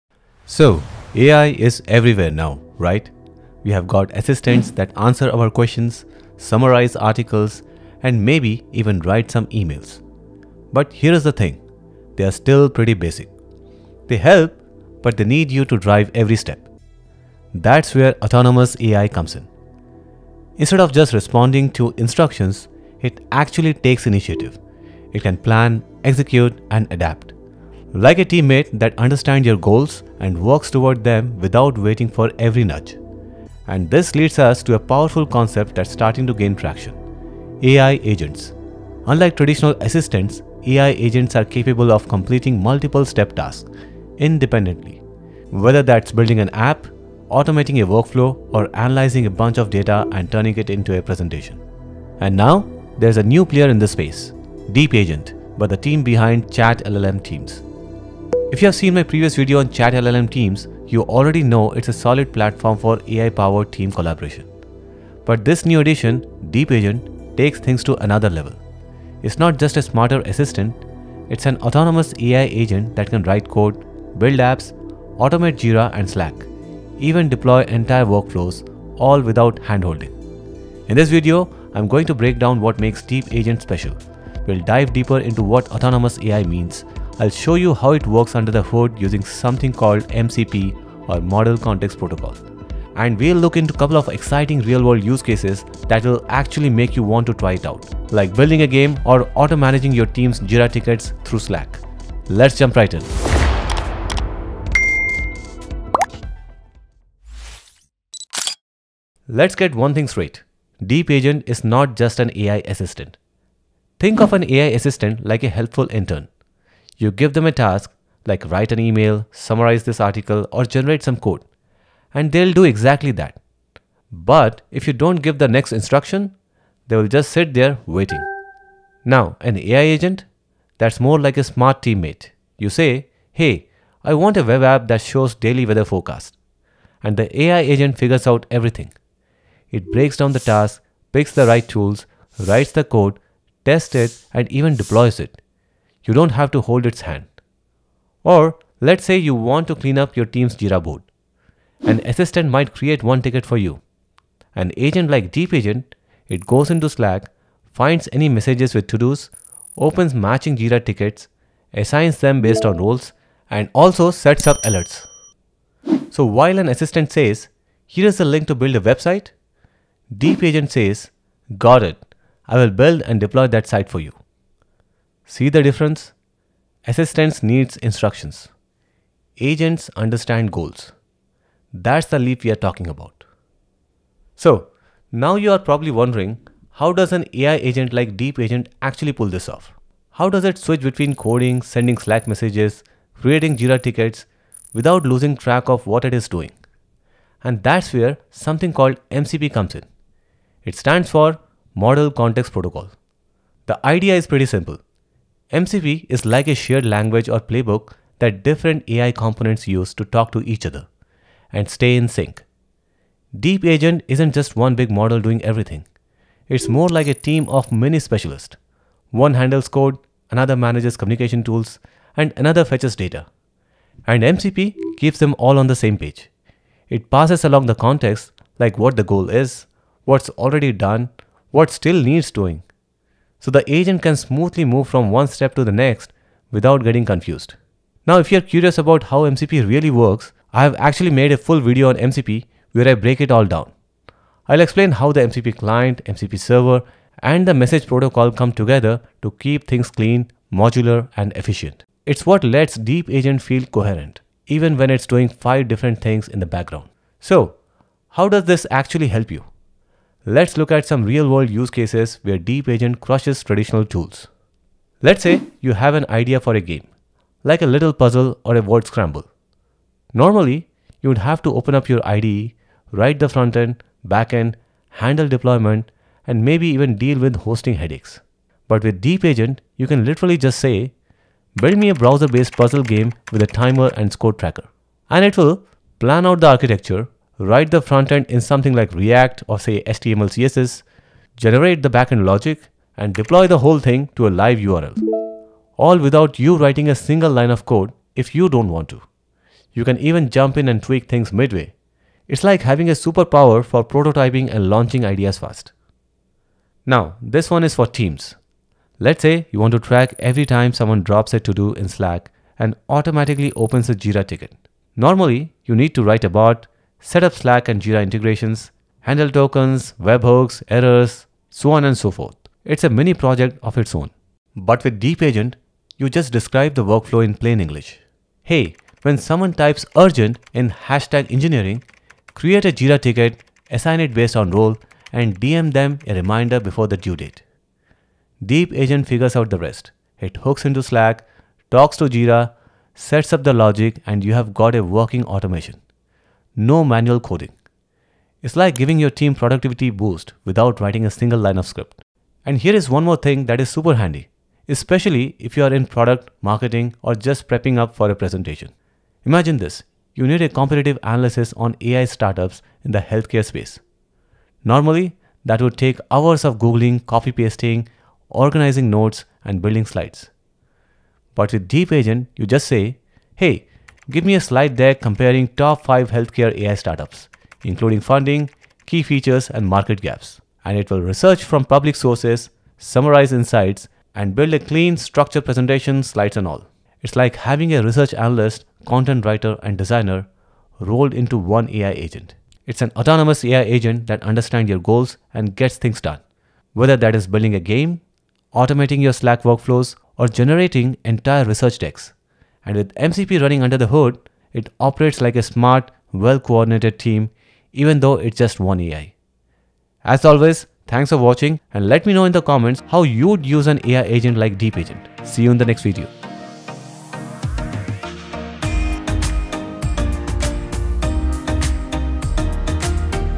I Tested DeepAgent: How AI Agents Use MCP to Think 🤖
I Tested DeepAgent, How AI Agents Use MCP to Think AI assistants like ChatGPT are amazing, but what if your AI could think, plan, and act without waiting for your next instruction? 🚀 Meet DeepAgent — the next-generation Autonomous AI Agent that builds web apps, automates Slack + Jira workflows, and creates research presentations with almost no human intervention. Powered by the cutting-edge Model Context Protocol (MCP), DeepAgent coordinates complex tasks like a smart, self-managed team. In this video, we break down how DeepAgent is different from traditional assistants, how MCP powers its multitasking abilities, and walk through real-world examples that will blow your mind — from launching live games to automating team communications. If you're excited about the future of AI Agents, Autonomous Systems, or building with AI, this is a video you don't want to miss. 🔥.

📌 Timestamps
000:00 – Introduction: AI Beyond Chatbots 🚀
00:49 – What is DeepAgent? (Revolutionizing AI Agents)
02:35 – Autonomous AI vs Traditional Assistants 🔥
03:20 – How DeepAgent Works: MCP (Model Context Protocol) 🧠
04:22 – Real World Example: Automating Jira, Slack, App Deployment
06:00 – Why DeepAgent Matters for Developers & Businesses